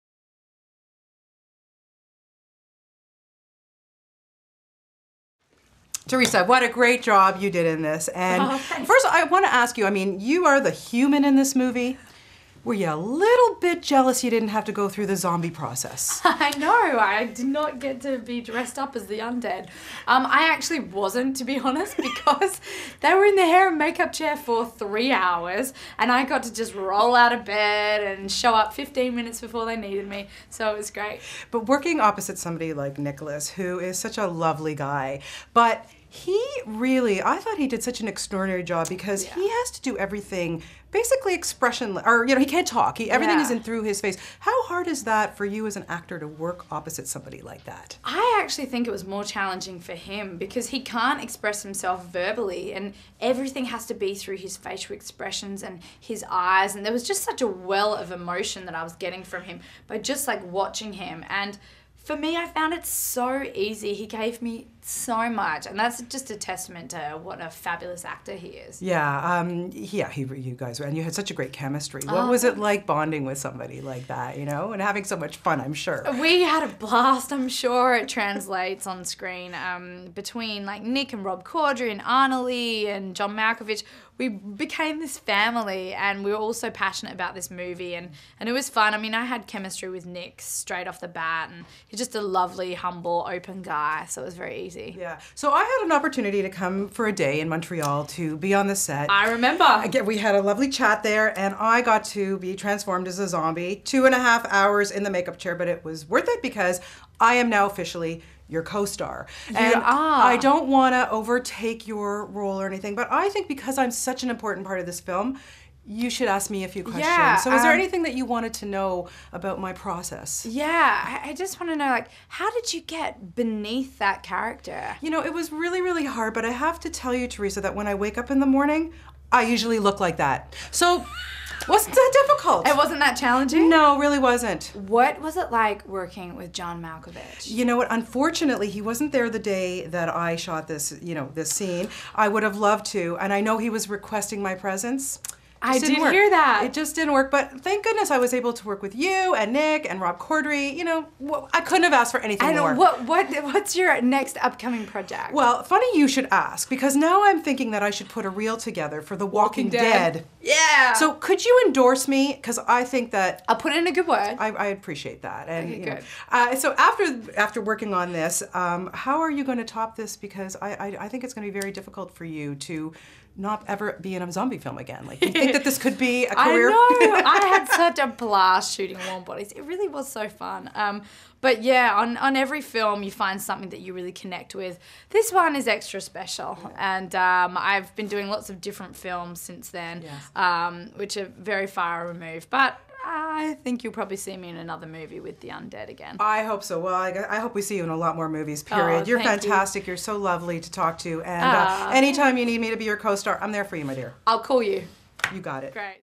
Teresa Palmer - Warm Bodies Interview HD
Teresa Palmer Interview, Warm Bodies - Australian beauty Teresa Palmer was really excited when she got the role of the heroine in the zombie rom-com Warm Bodies. Based on the best-selling novel, the film follows R (Nicholas Hoult), a zombie who falls in love with the non-zombie Julie (Teresa Palmer) after eating her boyfriend's brains.

Tribute.ca spoke with Teresa in Los Angeles about working opposite Nicholas Hoult and whether or not she was upset about not getting to go through the zombie makeup process.

Warm Bodies movie synopsis:

When a highly unusual zombie rescues a girl from imminent death at the hands of his cohorts, their unlikely romance sets in motion a chain of events that will transform him, his fellow dead, and maybe even the whole lifeless world.

Starring: Nicholas Hoult, Teresa Palmer, Rob Corddry, Dave Franco, Analeigh Tipton, Cory Hardrict, John Malkovich

In Theatres: February 1, 2013